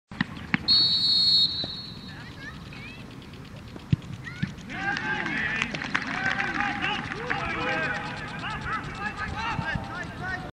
Handelfmeter Post SV im Testspiel gegen Dergahspor Nürnberg am 03.07.2014
Der Post SV Nürnberg vergibt im Testspiel gegen Dergahspor Nürnberg einen Handelfmeter (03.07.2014).